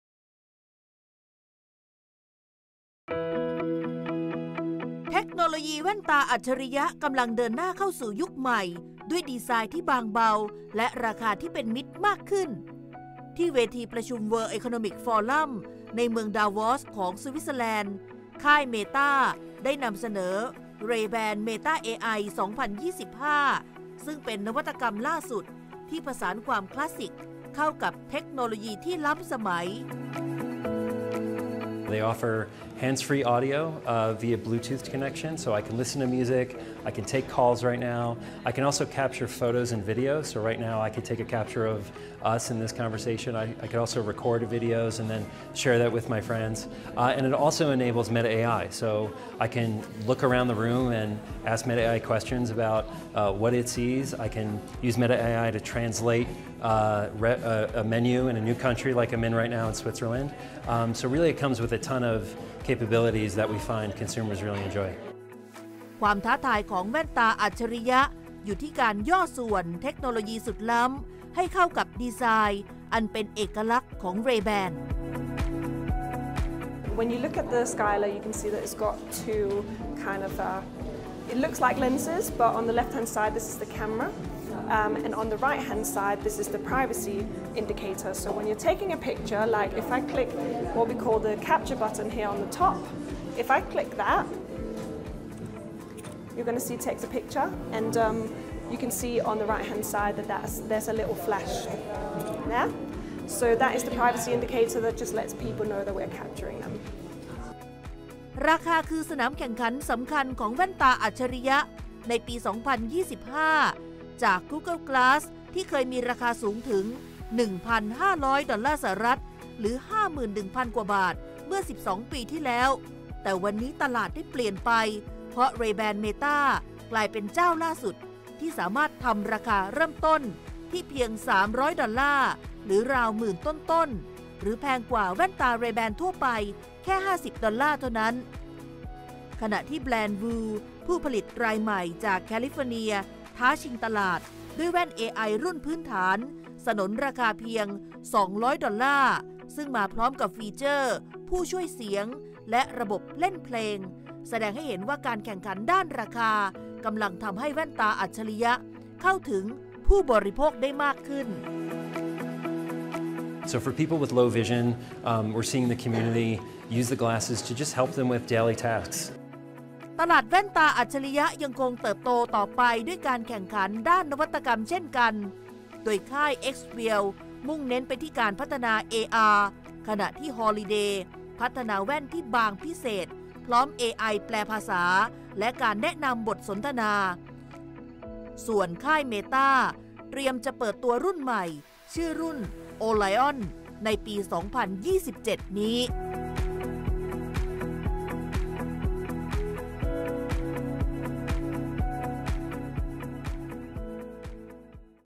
"แว่นอัจฉริยะ 2025" ล้ำได้แต่ไม่แพง
จากอุปกรณ์ไฮเทคราคาแพง สู่ “แว่นตาอัจฉริยะ” ที่ทุกคนเข้าถึงได้ นี่คือการปฏิวัติวงการแว่นตาอัจฉริยะครั้งสำคัญ